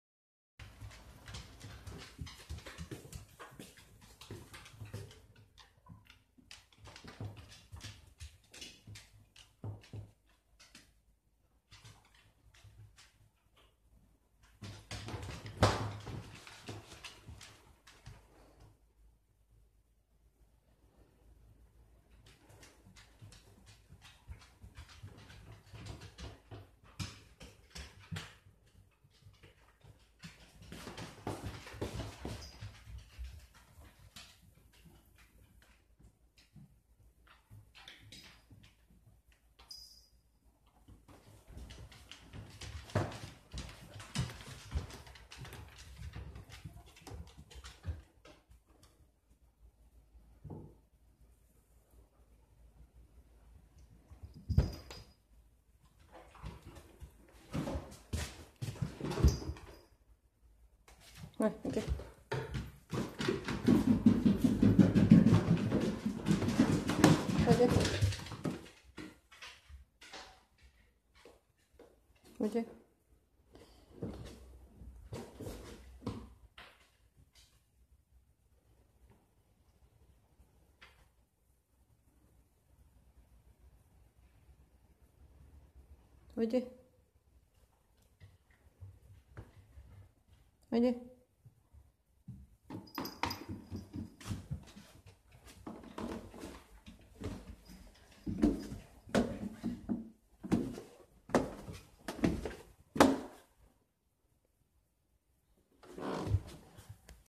何となく、団体行動
ありがちな犬達の団体行動に、なぜか猫が…。我が家の犬みたいな猫の名は、オニギリなのでした。